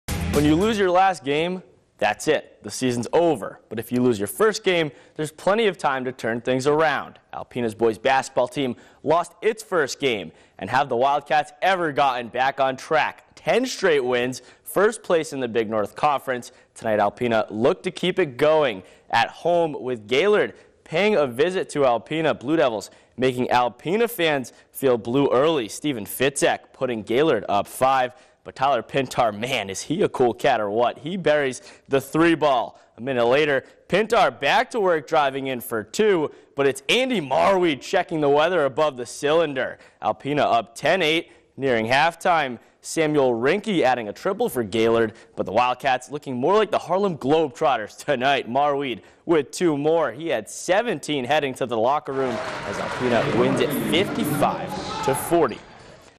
Boys Basketball: Gaylord at Alpena
Alpena boys win their 11th straight game, topping Gaylord.